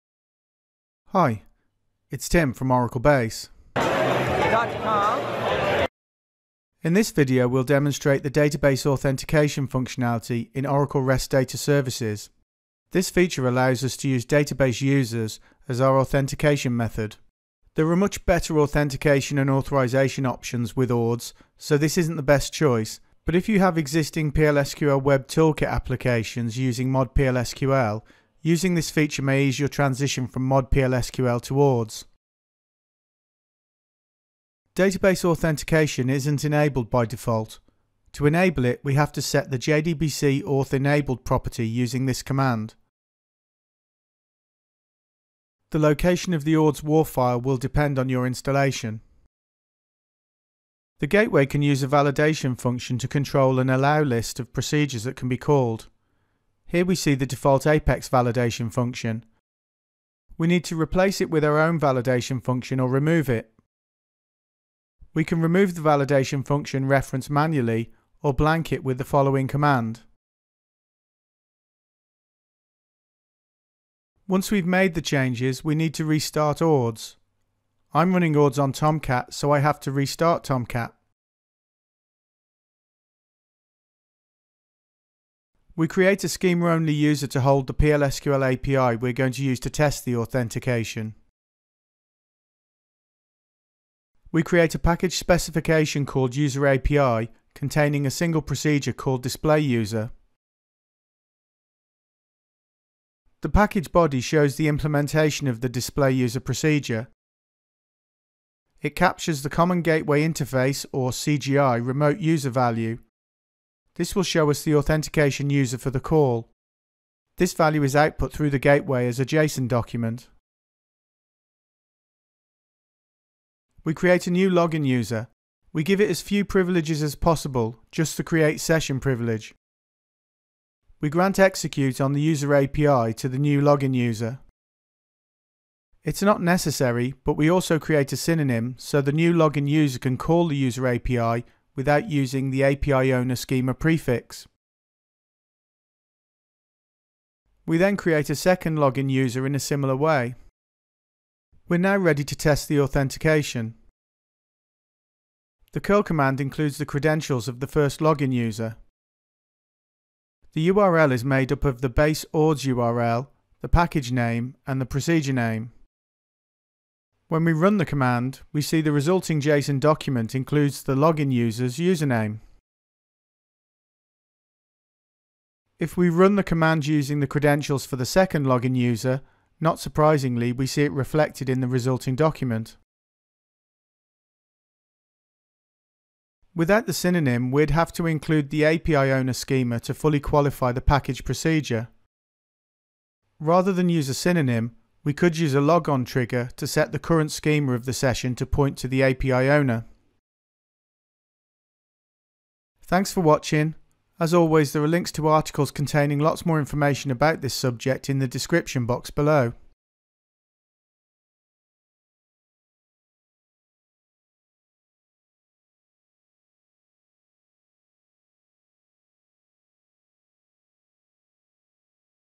Oracle REST Data Services (ORDS) : Database Authentication
In this video we'll demonstrate the Database Authentication functionality in Oracle REST Data Services (ORDS).